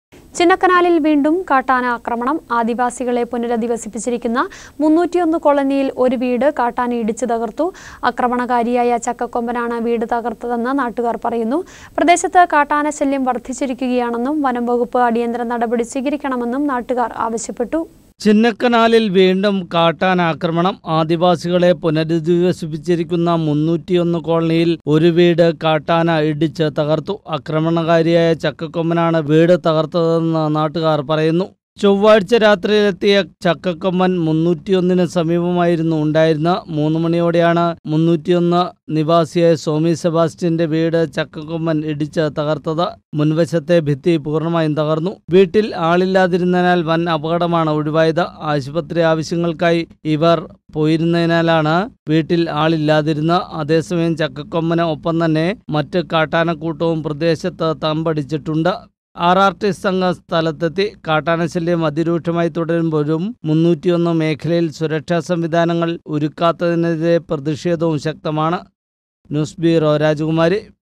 ചിന്നക്കനാലില്‍ വീണ്ടും കാട്ടാന ആക്രമണം
ചിന്നക്കനാലില്‍ വീണ്ടും കാട്ടാന ആക്രമണം. ആദിവാസികളെ പുനരധിവസിപ്പിച്ചിരിക്കുന്ന 301 കോളനിയില്‍ ഒരു വീട് കാട്ടാന ഇടിച്ചു തകര്‍ത്തു. അക്രമണകാരിയായ ചക്കക്കൊമ്പനാണ് വീട് തകര്‍ത്തതെന്ന് നാട്ടുകാര്‍ പറയുന്നു. പ്രദേശത്ത് കാട്ടാനശല്യം വര്‍ദ്ധിക്കുകയാണെന്നും വനംവകുപ്പ് അടിയന്തര നടപടി സ്വീകരിക്കണമെന്നും നാട്ടുകാര്‍ അവശ്യപ്പെട്ടു.